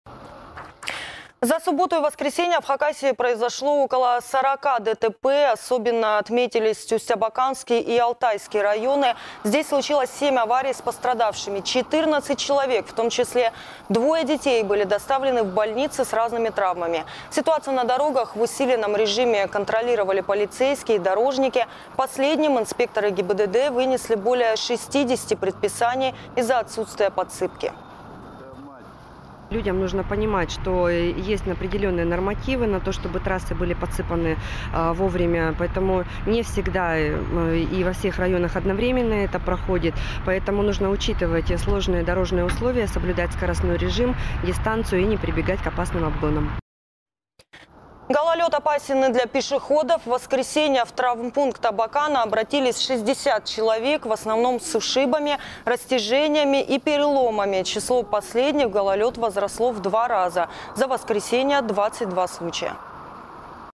За субботу и воскресенье в Хакасии произошло около сорока ДТП. 26.11.2018
Особенно отметились Усть-Абаканский и Алтайский районы.